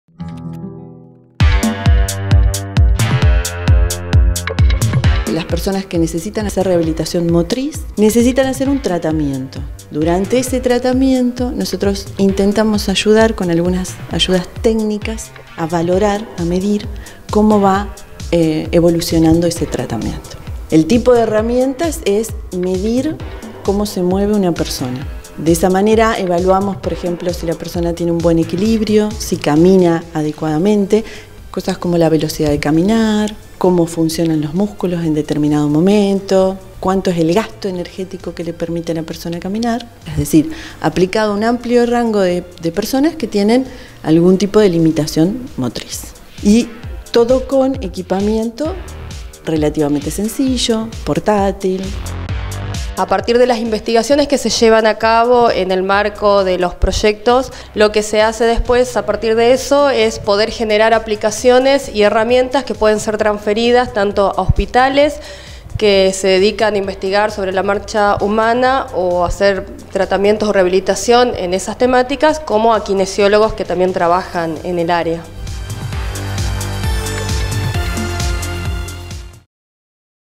Laboratorio de Investigación del Movimiento Humano
La dificultad física para realizar actividades cotidianas, como desplazarse de un lugar a otro, puede irrumpir en la vida de las personas por distintas causas y con diferentes grados de complejidad. En torno a los procesos de rehabilitación de esas funciones es que se trabaja en el Laboratorio de Investigación del Movimiento Humano (LIMH) de la Facultad de Ingeniería de la UNER. El objetivo es ayudar a los profesionales de la salud en la evaluación de los procesos de recuperación de sus pacientes, poniendo a disposición artefactos más sencillos y económicos que los ya existentes.